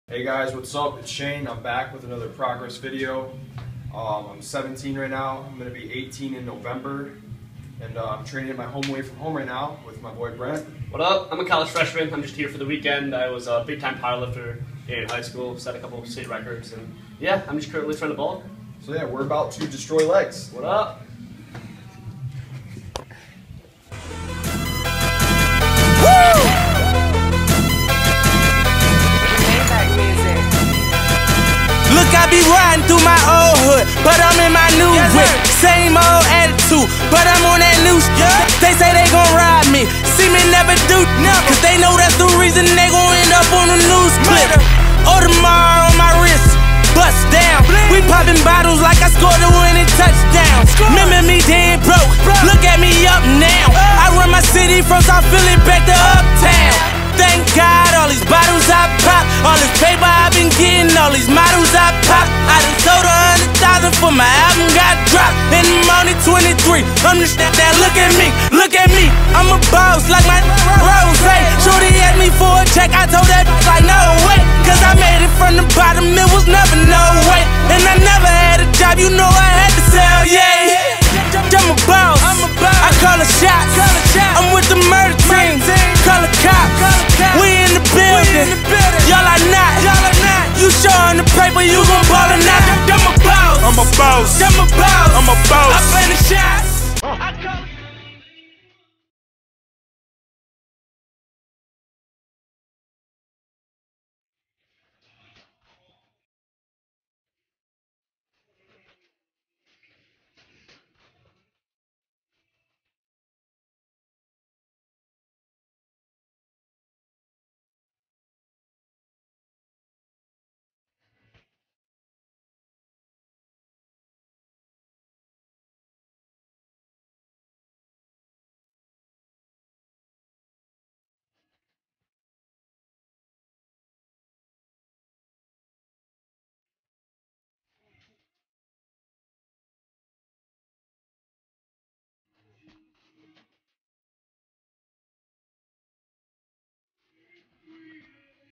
Shane Hodges: aesthetics
Keeping the dream alive.                                                                     Staying humble, hoping to step on stage with in the next year. I'm currently weighing 163lbs. Bench-245 Dead lift-325 Parallel squat-315 I suffered a shoulder injury awhile back and I'm slowly bringing my dead lift and bench up. I do not hold ownership to any of the  audio/music played in this video.                                                Instagram: Shodges40